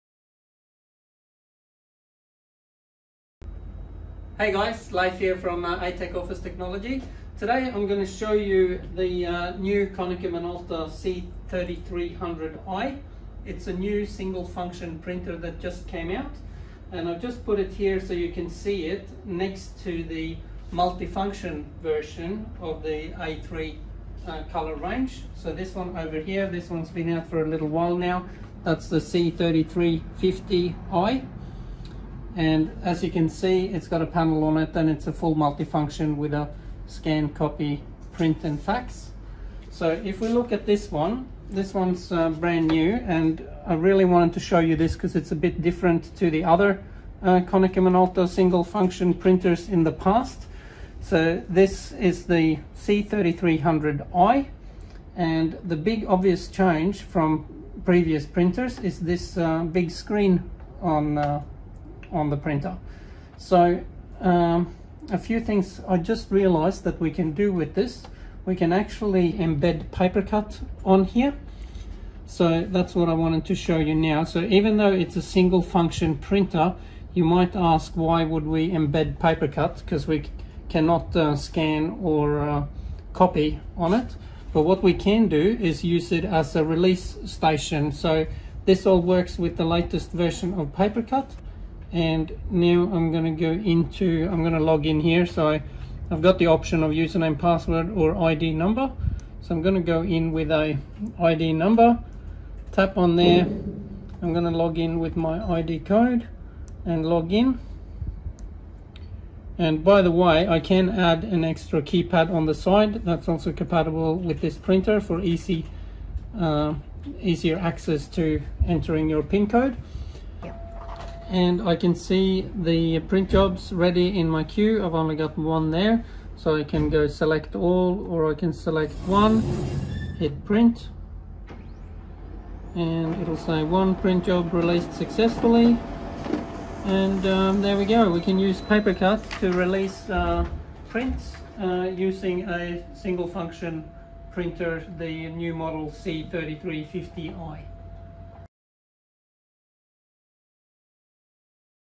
Konica Minolta C3300i & PaperCut Print Release
How to use PaperCut Print Release on Konica Minolta C300i Single Function Printer